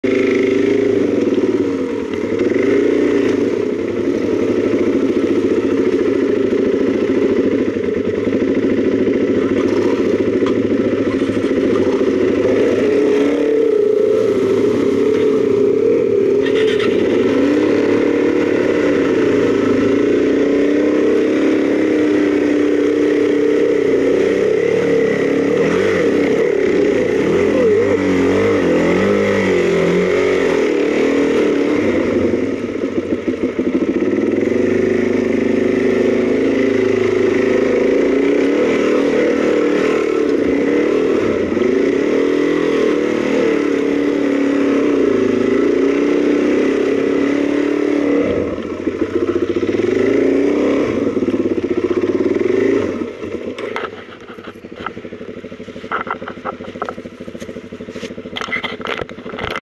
[2015/08/19 12:03] River Crossing in Old Telegraph Track, Cape York (4K)
[18–23 Aug 2015] Cape York Motrcycle Tour (The Top to Cairns Southbound) with Outback Adventure Treks.